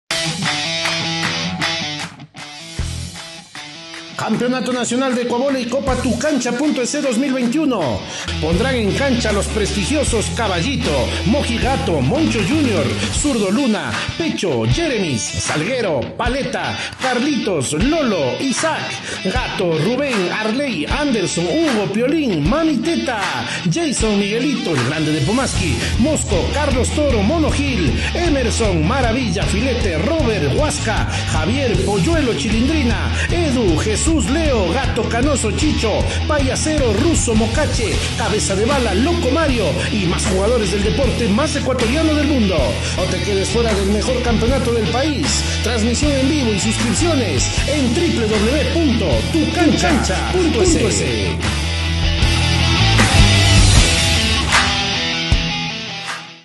Campeonato Nacional de Ecuavoley Copatucancha.ec 2021 .
Sigue de cerca este emocionante campeonato, apoya a tu equipo y el deporte 100% Ecuatoriano, por cada suscripción estarás aportando para el obsequio de Balones a escuelas de Ecuavoley. Síguelo de cerca y vive la emoción solo por Tucancha.ec .